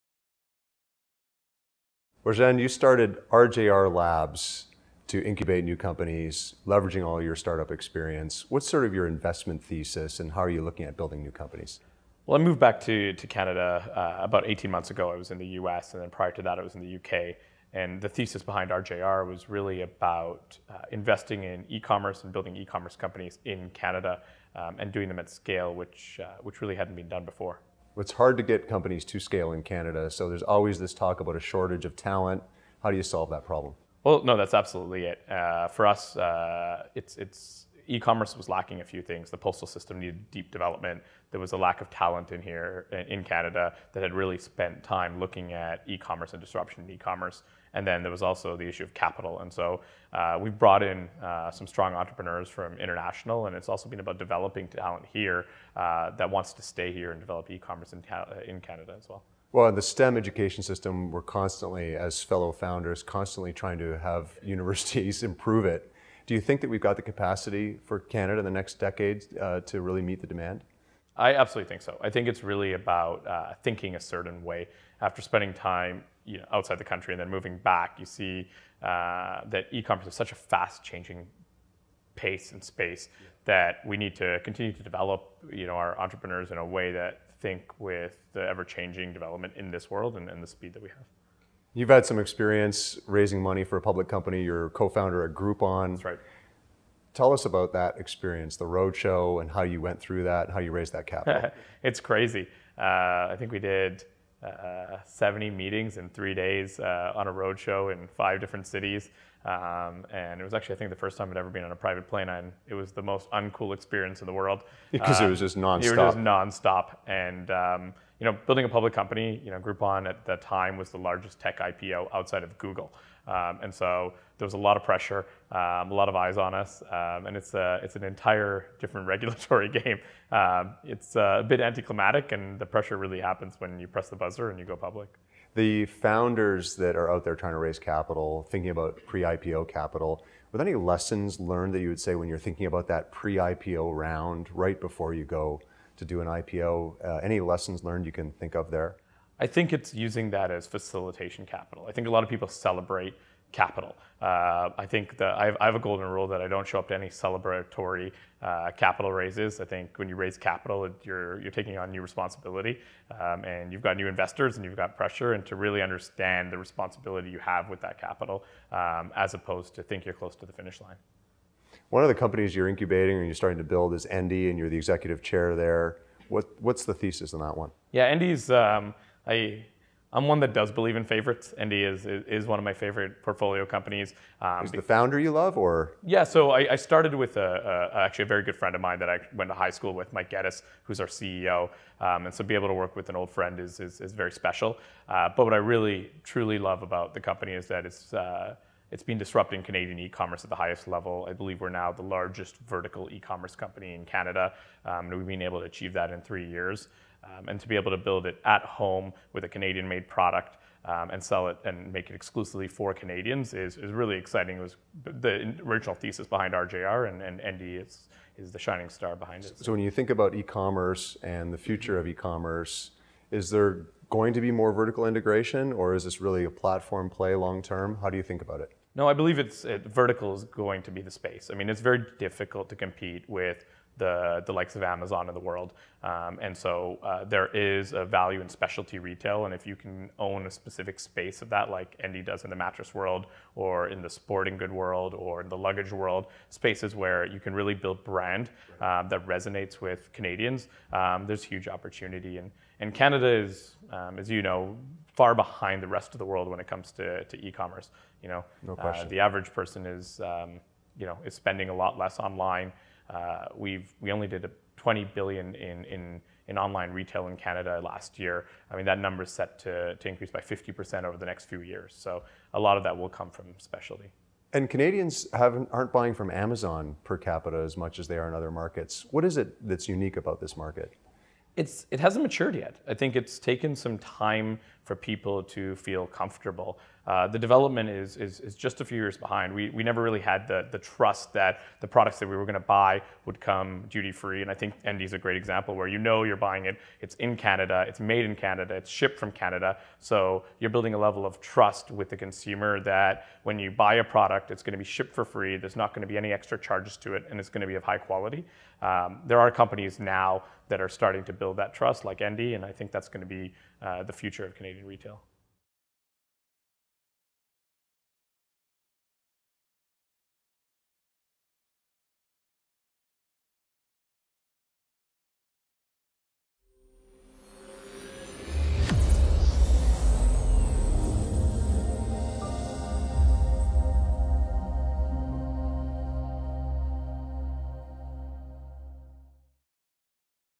Specialty Retail
Rajen Ruparell, Co-Founder and Chairman of Endy shares with Anthony Lacavera what he looks for when investing in a startup and why he is focused on building e-commerce companies in Canada. Rajen also explains how he views raising capital and lessons learned from taking Groupon public.